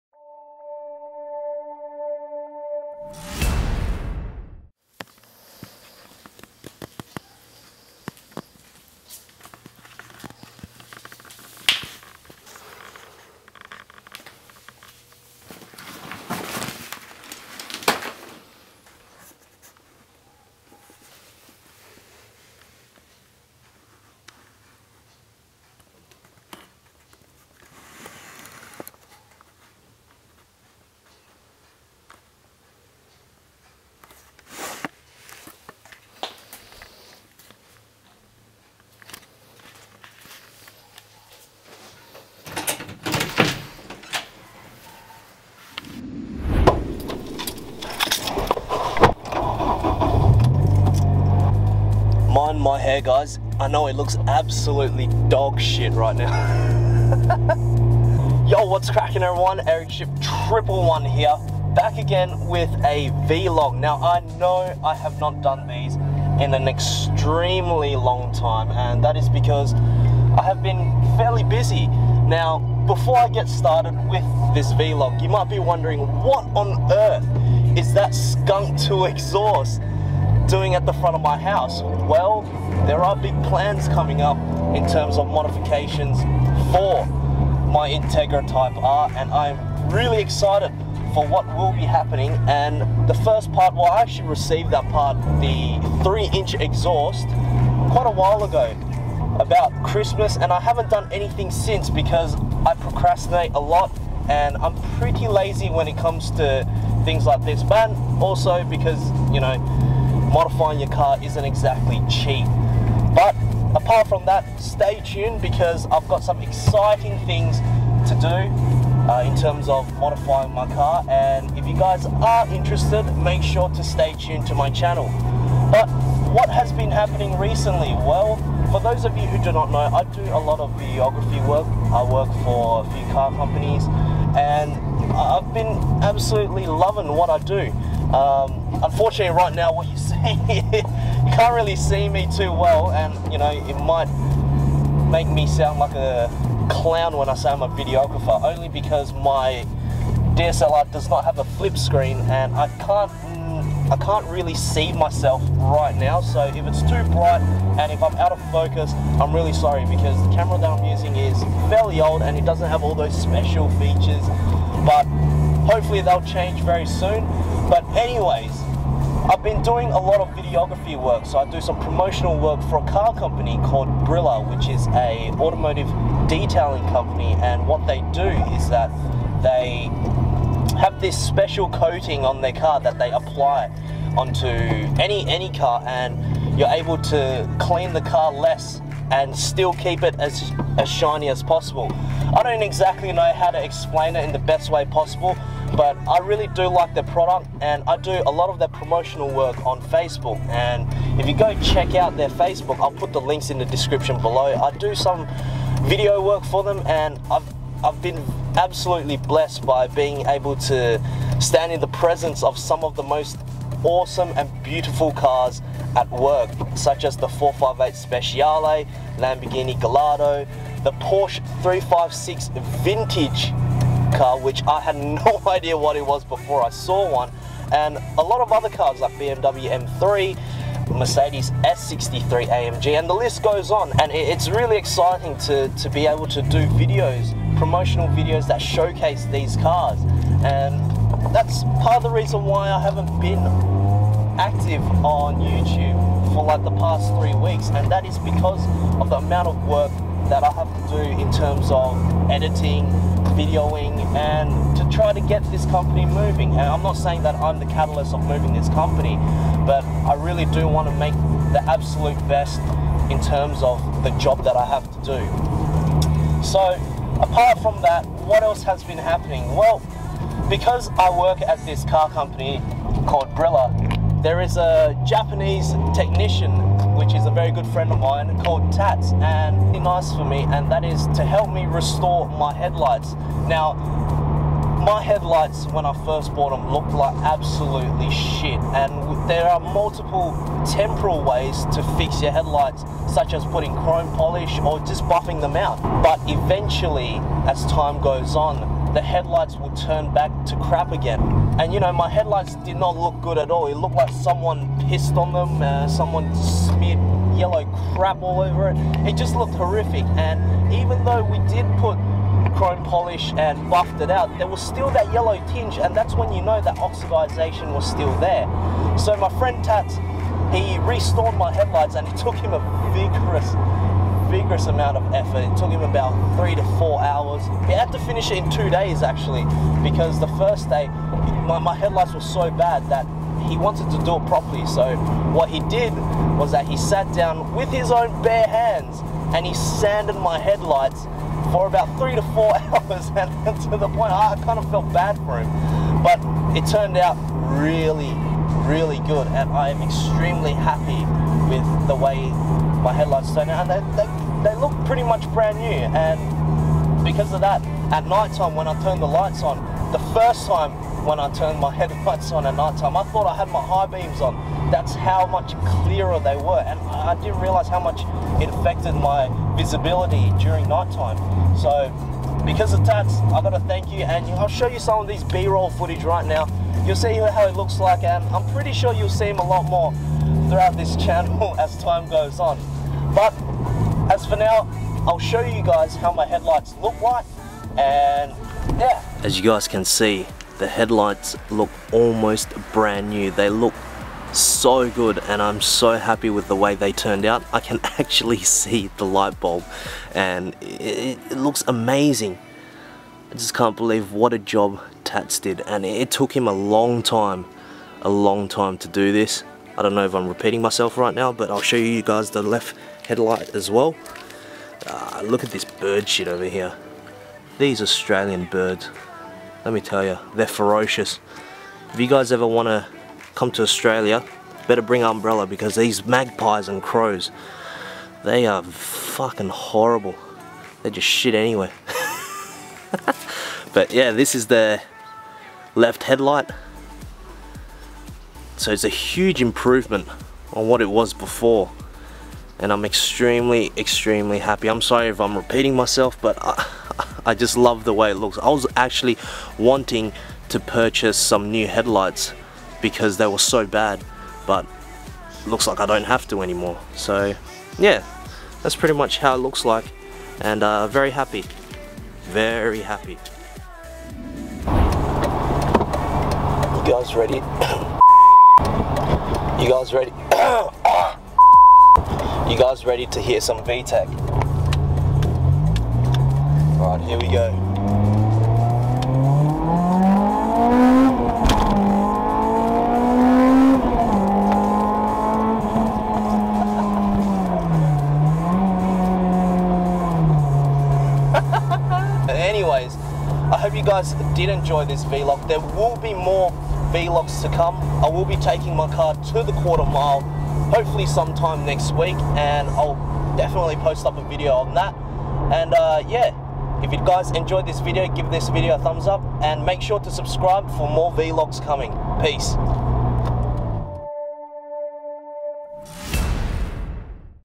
Honda Integra TYPE R DC5 - Headlights RESTORED FINALLY! | Vlog #3
Hey guys just a quick update video of what I have been up to and what will be happening to my Honda Integra Type R DC5 (Acura RSX-S). Enjoy!

- XBL: ericship

- PSN: ericship 111